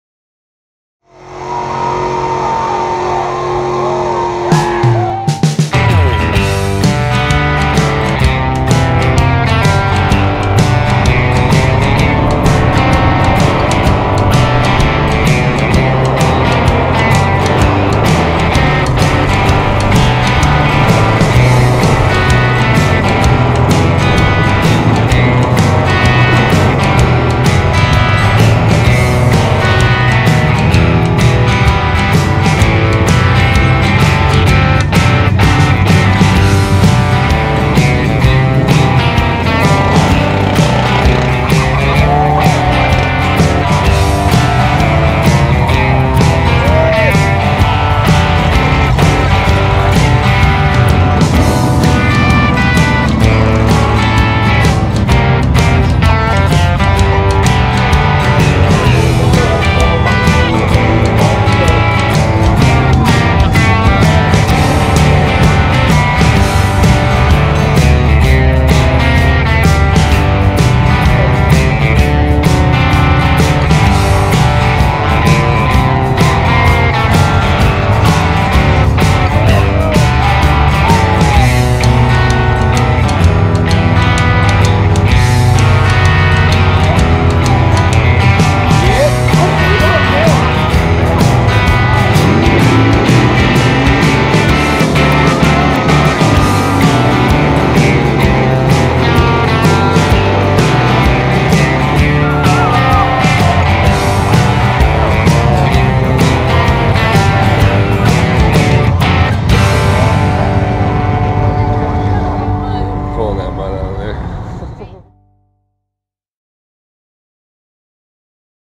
Michigan Mud Jam 2014
We had a great time checking out the Michigan Mud Jam in my Grandparents home town, can't wait to do it again next year now that we know what its all about.

Filmed on GoPro Hero3+ Black Edition